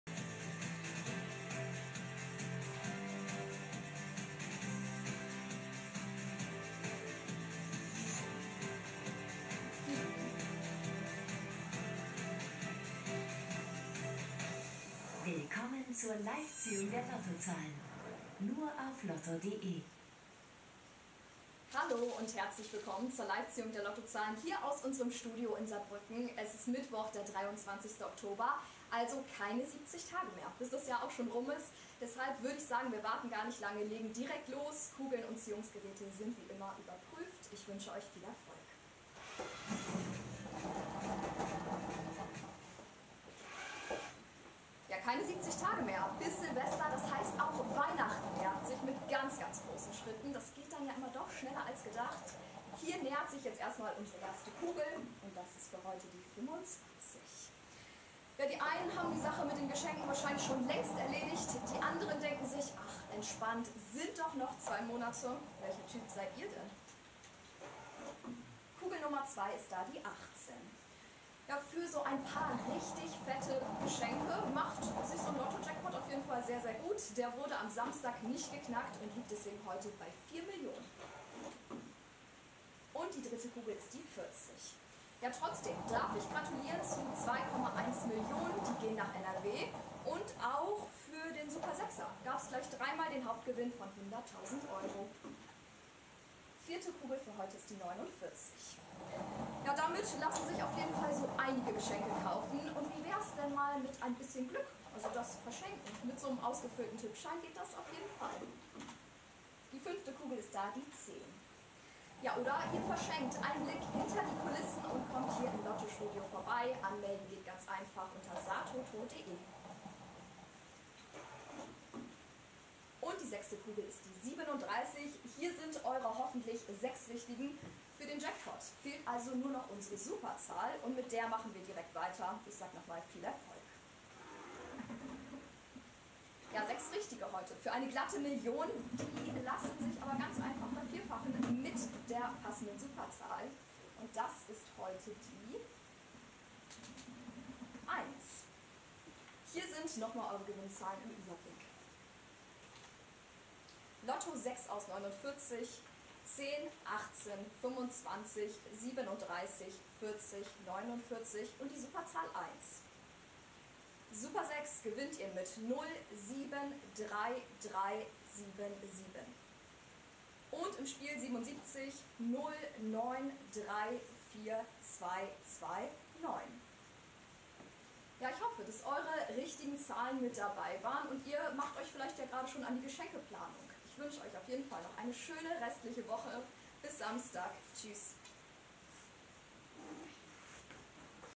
Die Ziehung der Lottozahlen vom 23.10.2024 in 360 Grad
Erleben Sie die Ziehung der Lottozahlen aus einer neuer Perspektive... in 360 Grad!
Die Navigation ist einfach: Sie können entweder über das Bedienfeld direkt im Video oder mit gedrückter Maustaste das Lottostudio erkunden. Noch einfacher geht es über die YouTube App und mit den Lagesensoren Ihres mobilen Endgerätes. Mit der +-Taste können Sie zudem in das Videobild hineinzoomen.

Für eine noch detailgenauere Darstellung empfehlen wir die Ansicht in 4K. Bitte beachten Sie, dass die 360°-Funktionalität nicht mit jedem Internetbrowser funktioniert.